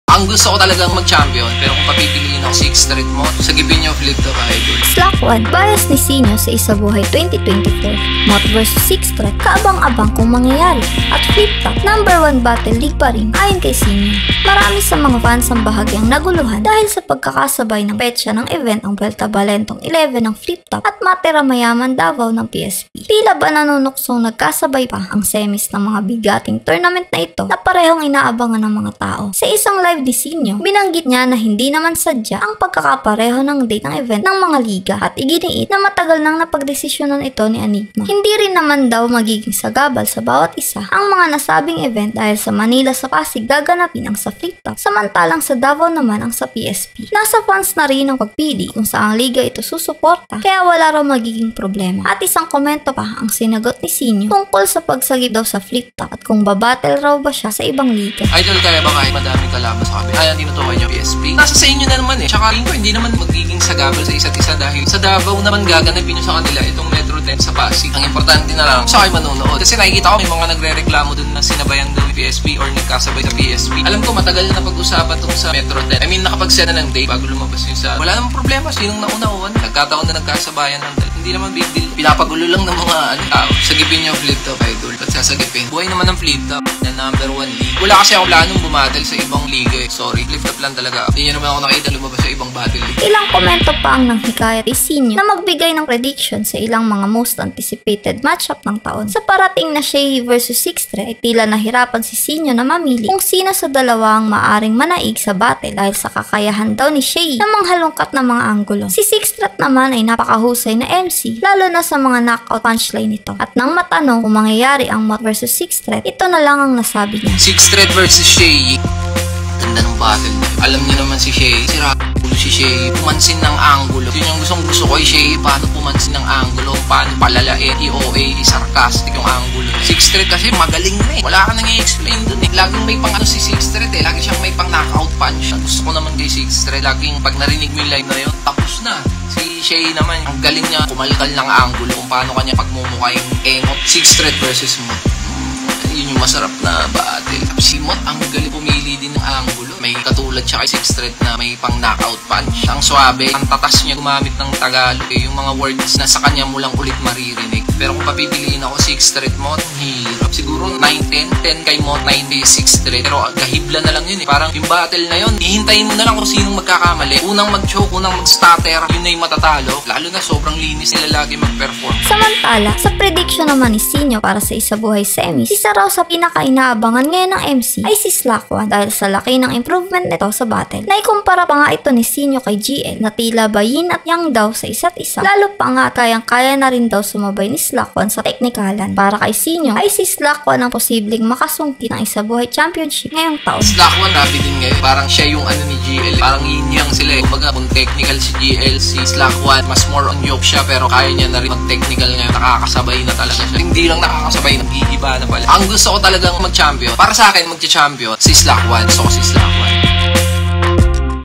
ICONS FEED HIP-HOP UPDATE AUGUST 20 2024 | SINIO | MHOT THOMAS \ SIXTH THREAT | FLIPTOP | SLOCK ONE
Prediction ni Sinio sa Mhot Thomas vs Sixth Threat sa Fliptop at kay Slock One..

𝗖𝗥𝗘𝗗𝗜𝗧𝗦
Narrator: Qatstyle